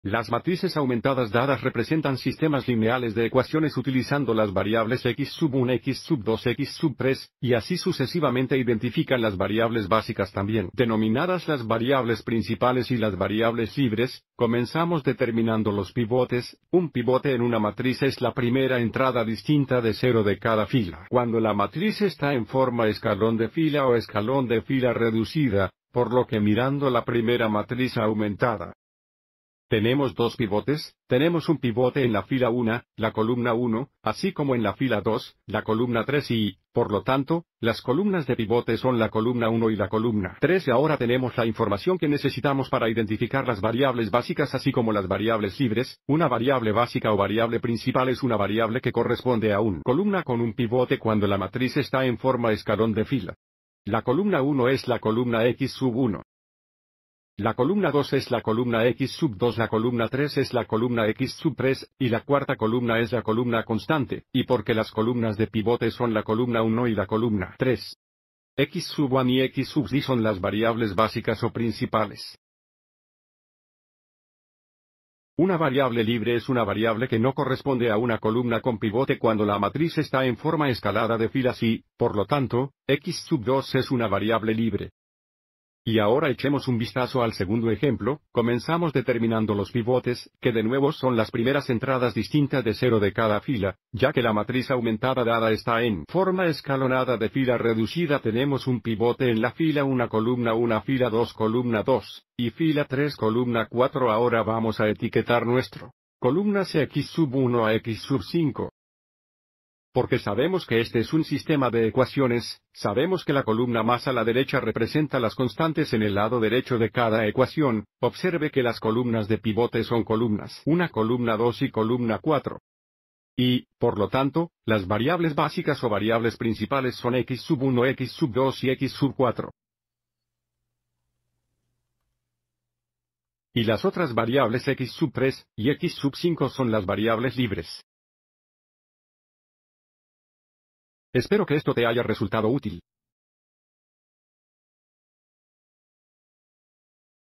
Determinar variables básicas (iniciales) y variables libres dada una matriz en RREF
Este vídeo explica cómo determinar las variables básicas y libres mediante la determinación de los pivotes y las columnas de pivote de una matriz aumentada es RREF.